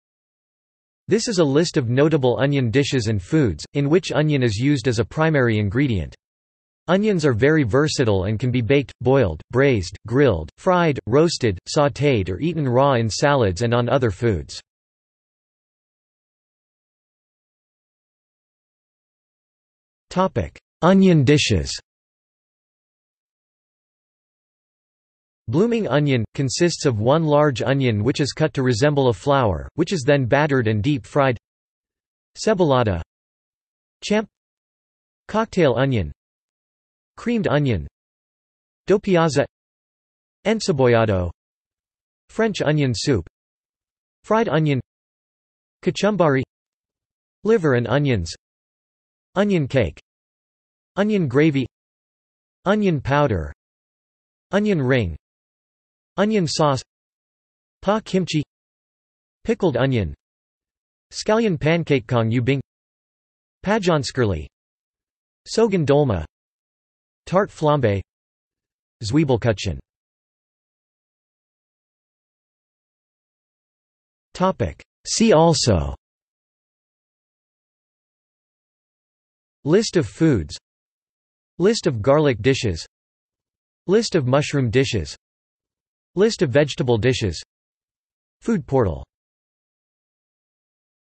List of onion dishes | Wikipedia audio article
This is an audio version of the Wikipedia Article:
List of onion dishes

00:00:16 undefined
00:01:10 undefined

- Socrates

SUMMARY
This is a list of notable onion dishes and foods, in which onion is used as a primary ingredient. Onions are very versatile and can be baked, boiled, braised, grilled, fried, roasted, sautéed or eaten raw in salads and on other foods.